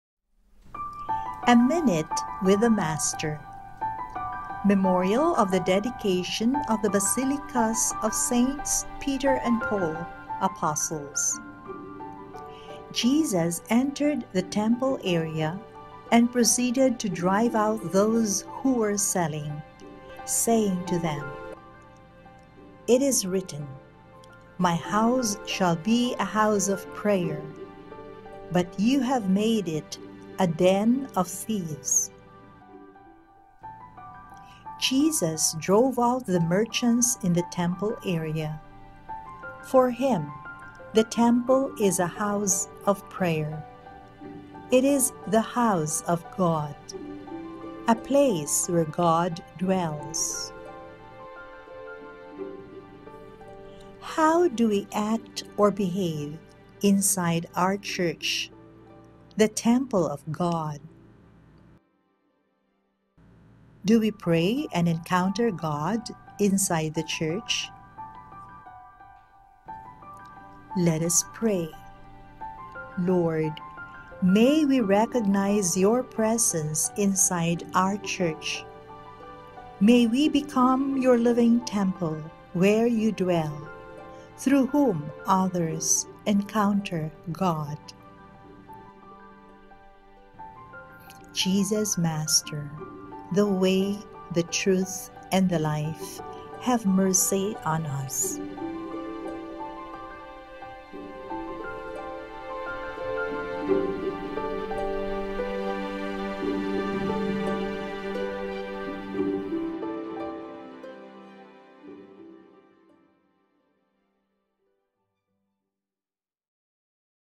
November 18, 2022
Jesus entered the temple area and proceeded to drive out
those who were selling, saying to them,  “It is written, My house shall be a house of prayer, but you have made it a den of thieves.” (Lk 19:46)